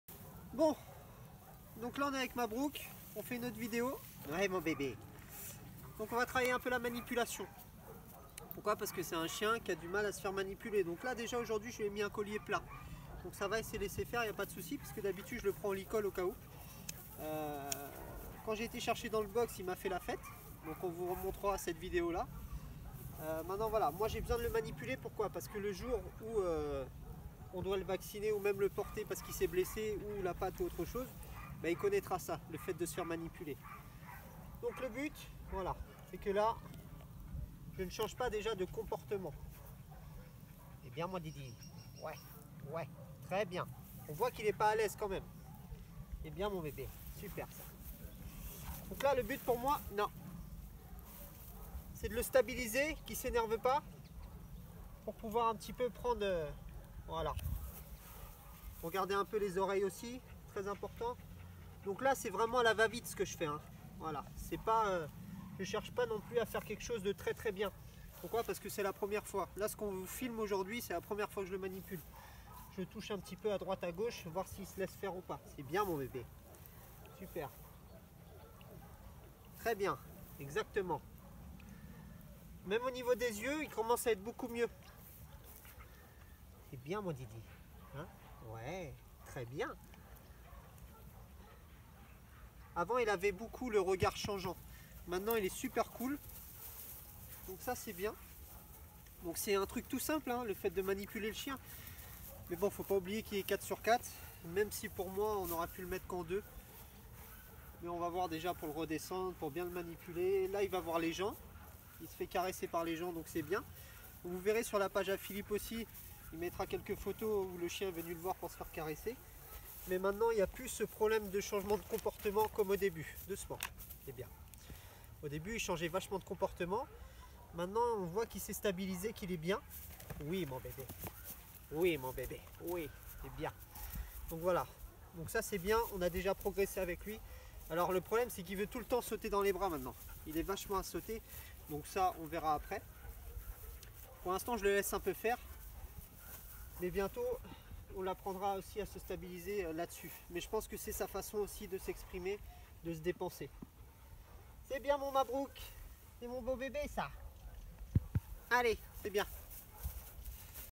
Mabrouck part 4 TRANSFORMATION 😁😁😁
Changement radical de Mabrouck, dans cette vidéo je lui apprend à se faire manipuler pour qu’il comprenne que la main humaine n’est pas un danger et pour travailler les soins plus tard